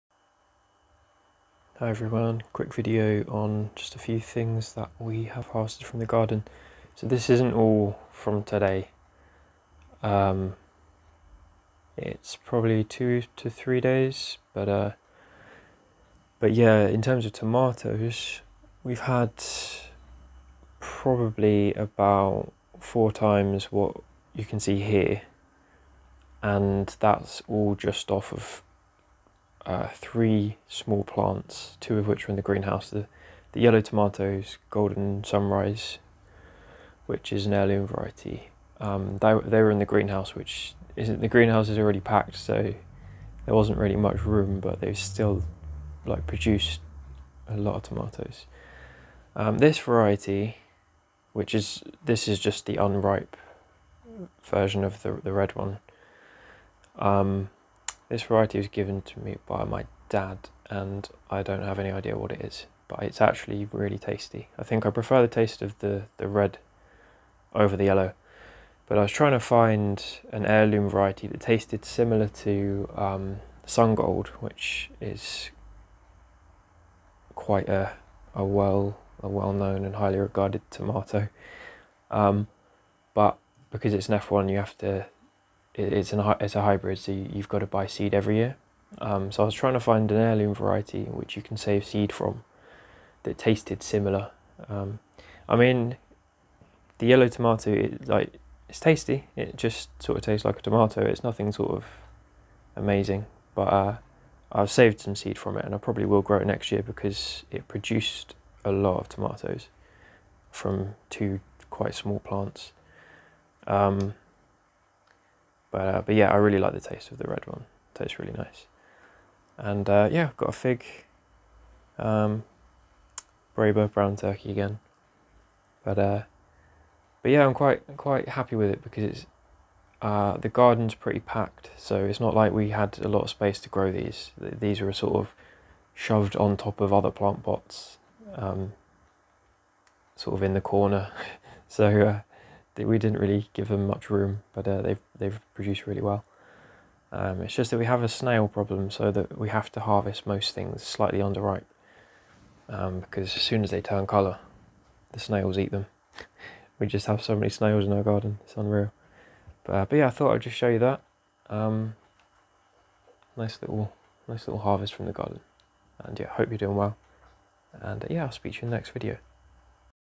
September harvest | tomatoes and figs
Small harvest of golden sunrise tomatoes, and unknown striped tomato and a brown turkey fig, from out small back garden here in Kent, UK